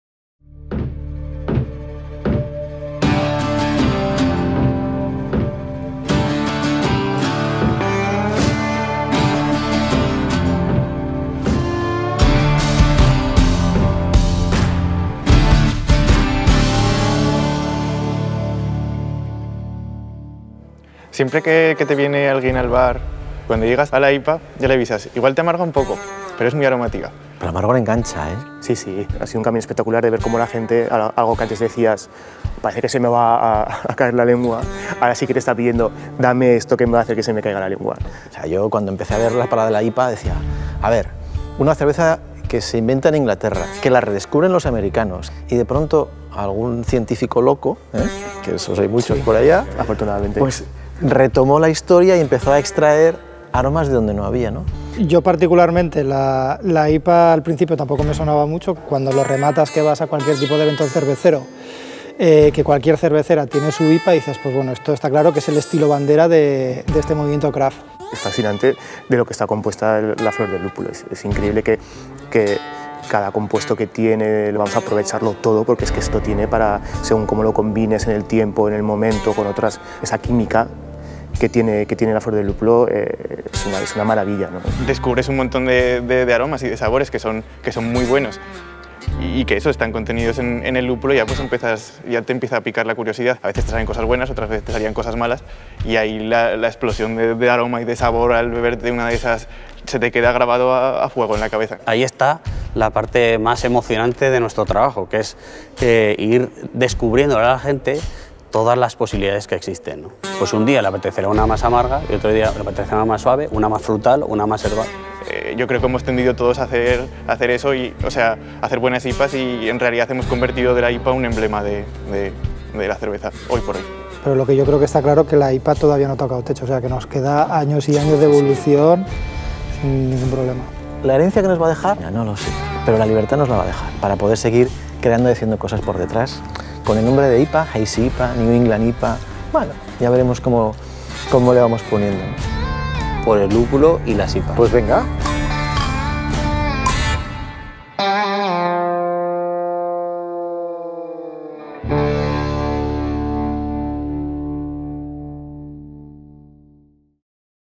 Video promocional | AMBAR doble IPA | DOSIS VIDEOMARKETING
Vídeo Promocional donde el maestro cervecero de @CervezasAmbar1900, Antonio Fumanal junto con otros cuatro maestros cerveceros aragoneses, nos descubren el mundo de las IPAs y su impacto en nuestro país.